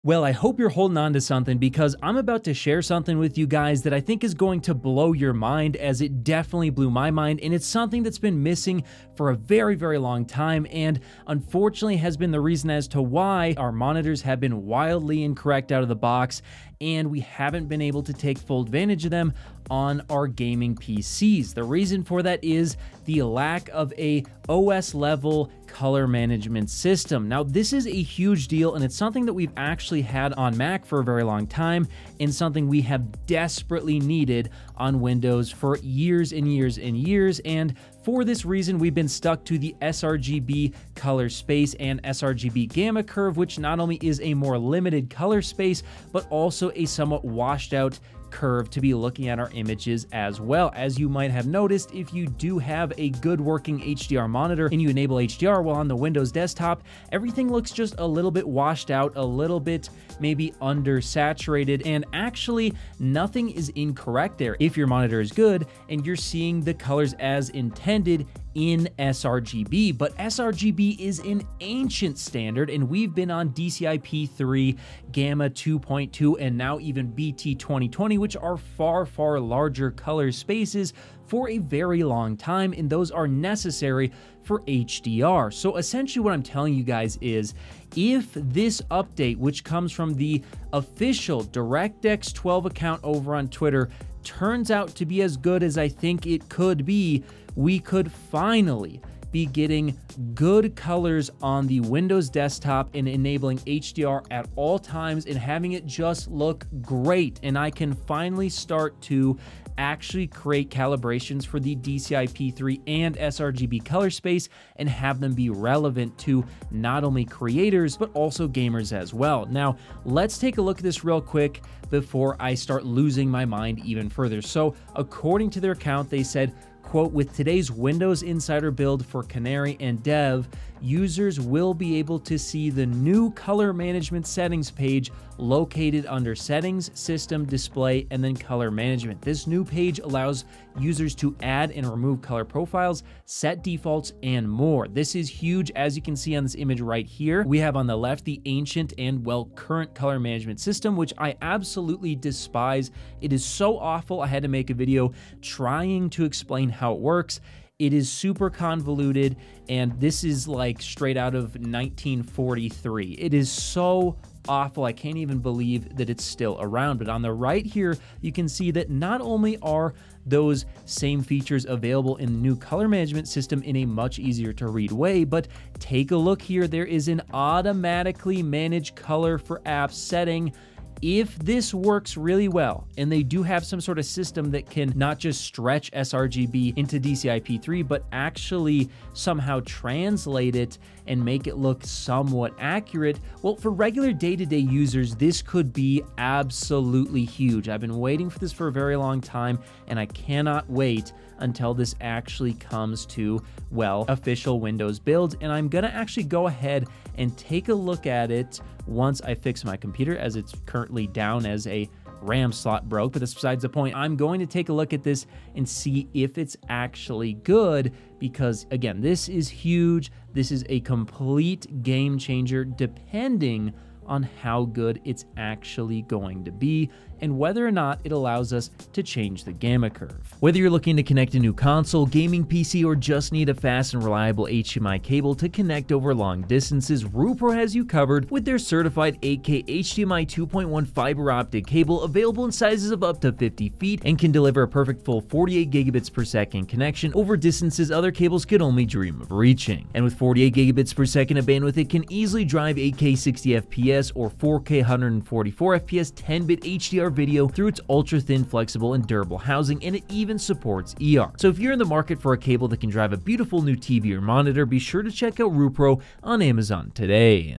Biggest OLED Upgrade Ever - HDR Monitor Update
This very well may be the biggest upgrade for HDR and OLED monitors ever.